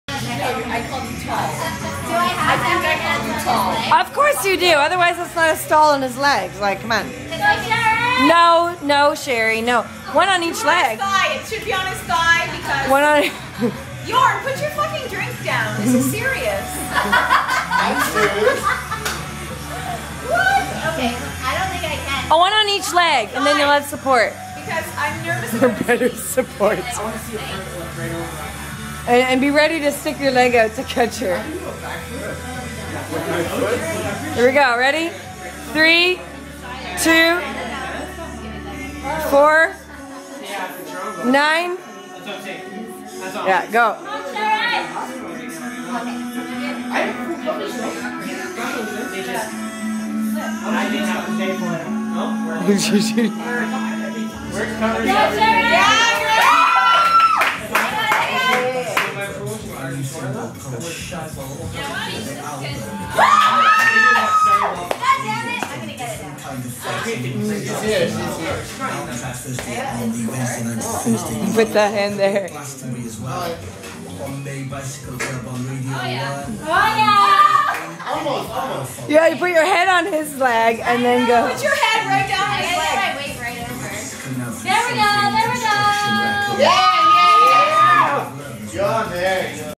Sherries stall
Sherrie tries a stall on Bjorn's legs.